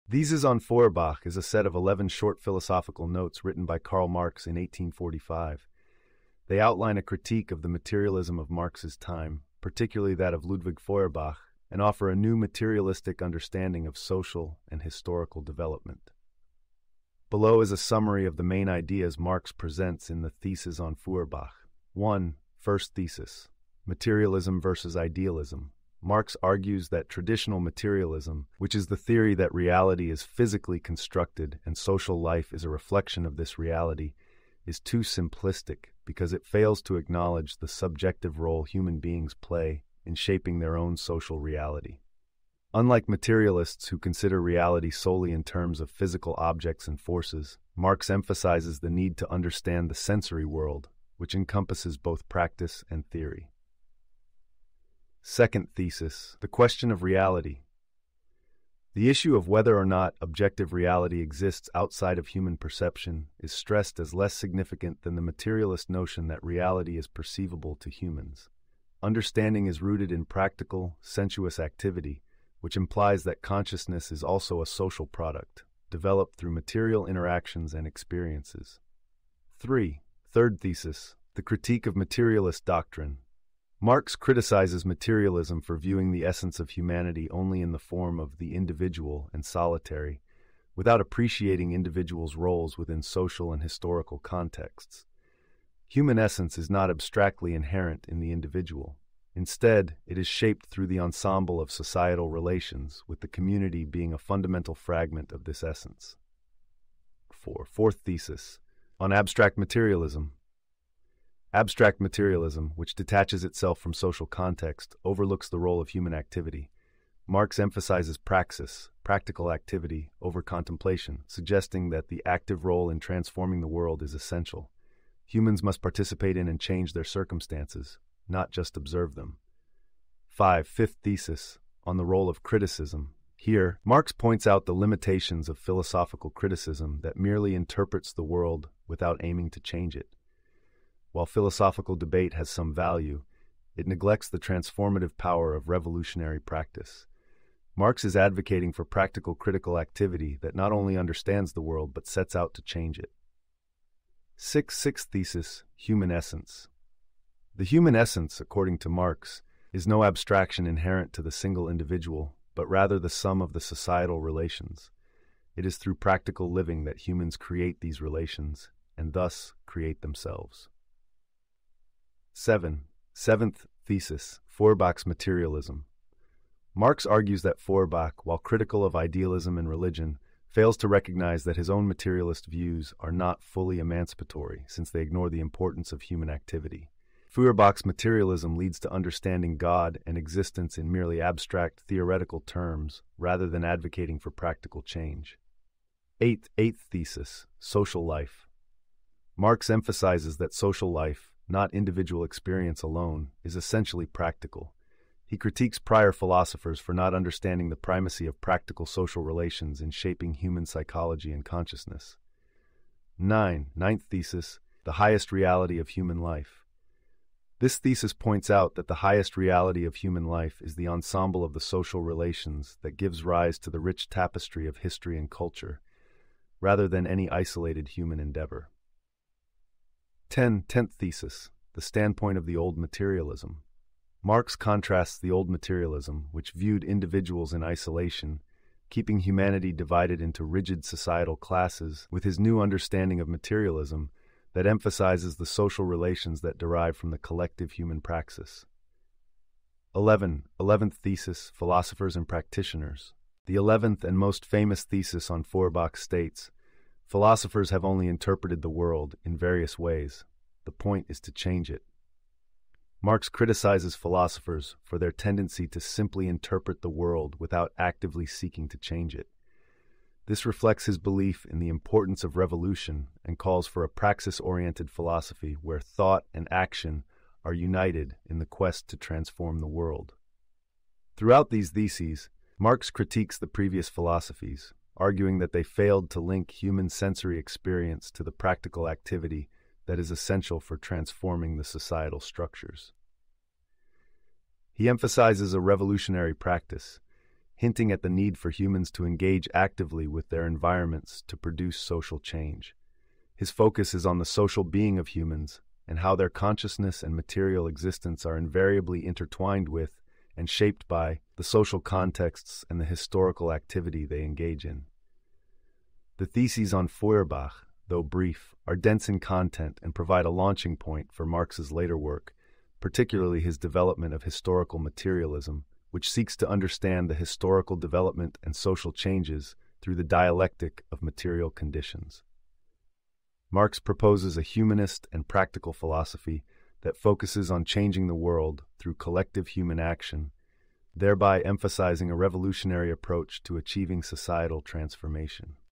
Karl Marx - Theses on Feuerbach (1845)
In "Theses on Feuerbach," Karl Marx criticizes philosopher Ludwig Feuerbach's materialism and advocates for revolutionary praxis, arguing that philosophers have only interpreted the world in various ways, but the point is to change it.